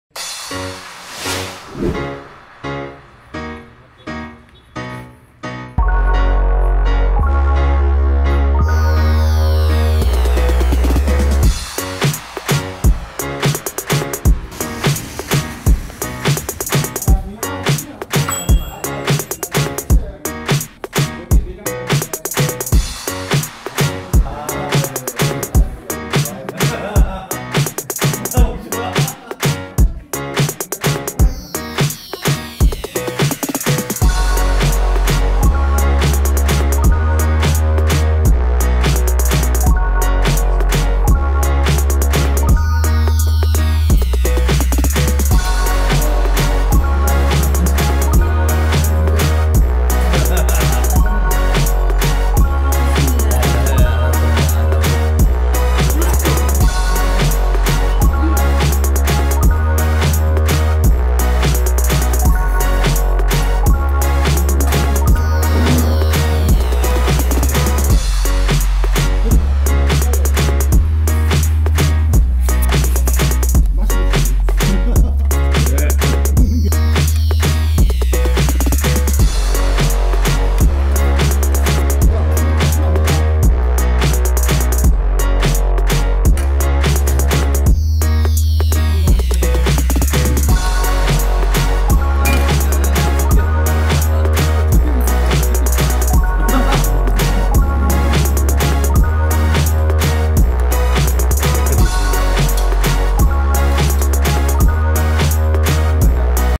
고성군맛집 생태탕 지리탕 전문 성진회관
강원도 고성군에 위치한 맛집
 이른 아침 아침식사를 하러 이곳에 방문하였습니다.
 바닷가가 보이는 전망좋은 집구경 및 해수욕장 등을 두루 두루 들렀는데요
오늘은 이곳 생태 지리탕 한번구경하시죠.